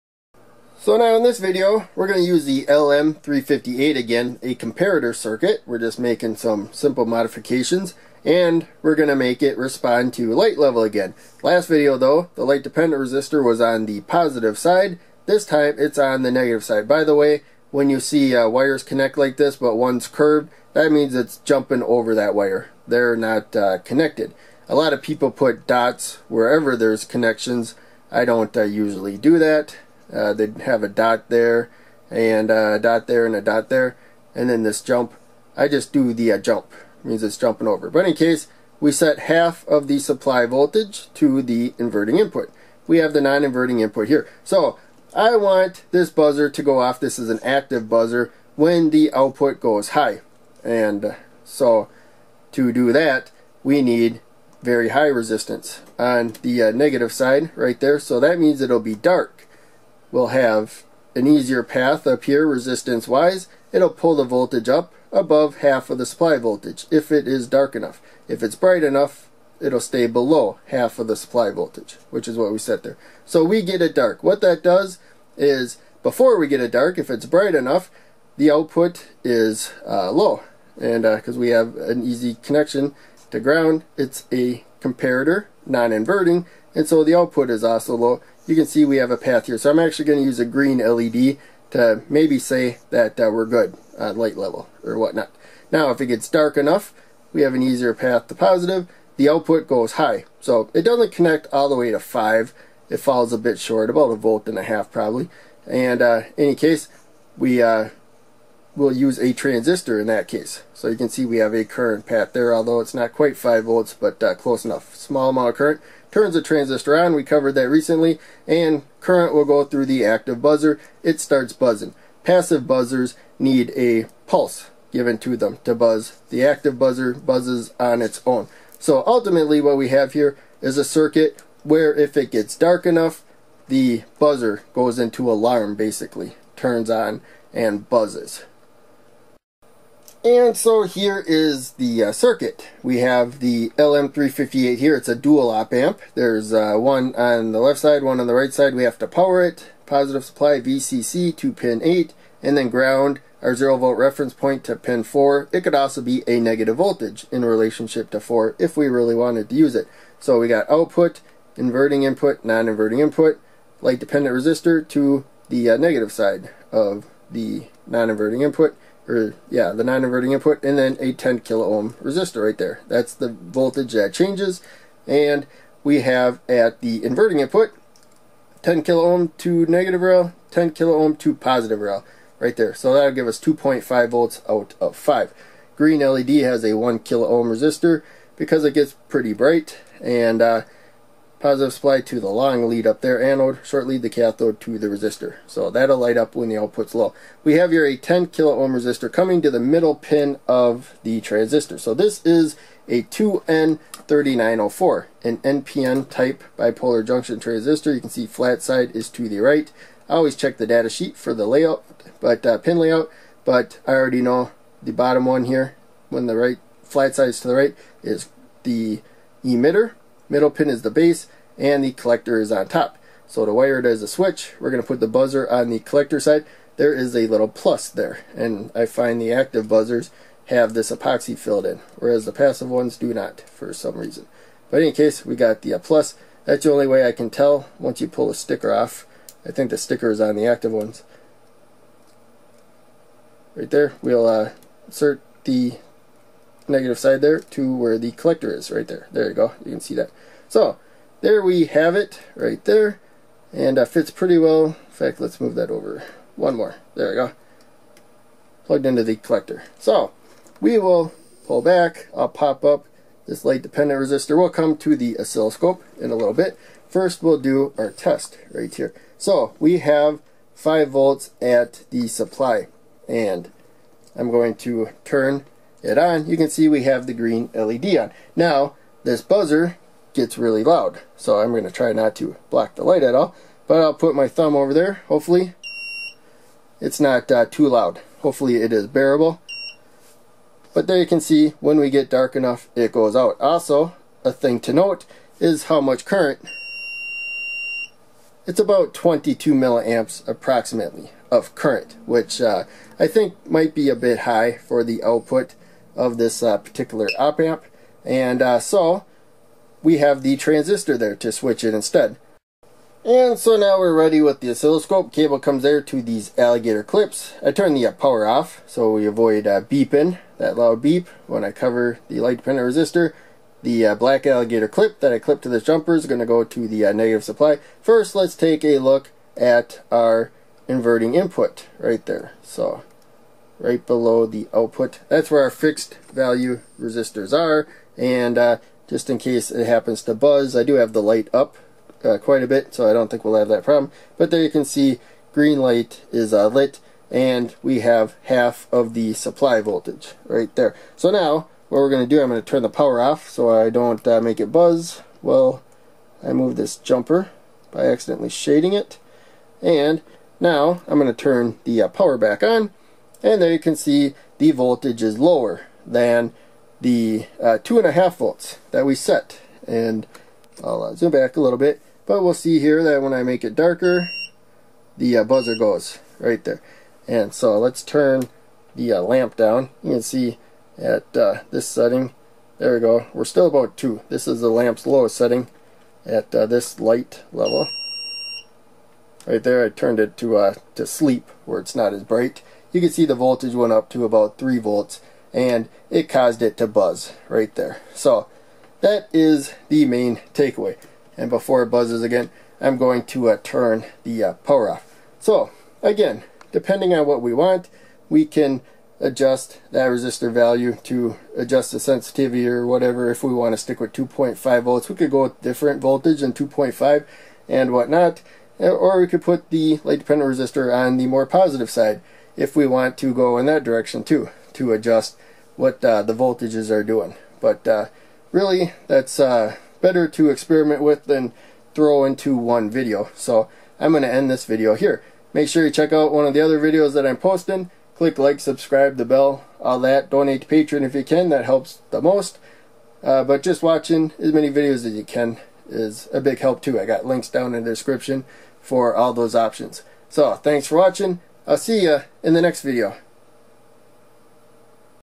Active buzzer low light alarm made with LM358 op amp comparator with LDR and 2N3904 NPN BJT circuit
Another use for the LM358 non inverting comparator circuit can be to turn a buzzer on when light levels get low enough. Not sure how well the LM358 output can power a buzzer, I used the output high signal to turn on a NPN bipolar junction transistor (BJT) wired as a switch in series with the buzzer.